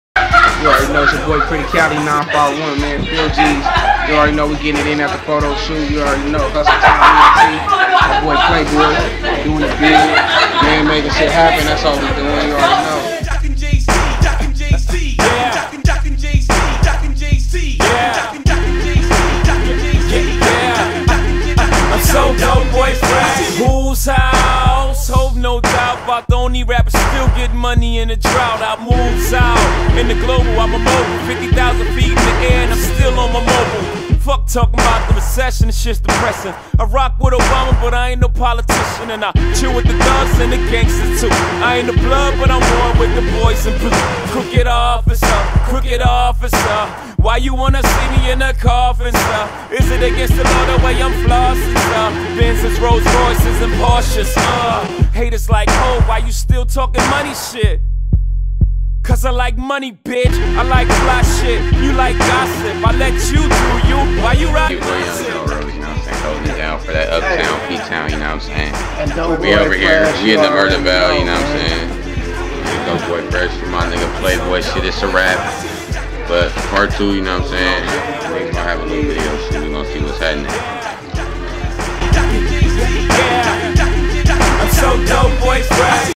HUSTLETIME ENT- JWILLIAMS PRODUCTION
On the set, and behind the scenes of the Hustletime Entertainment Photoshoot.

Hair: Stay fresh cutz
Makeup: Jasmine Reed

Hustletime Ent. Models & Artists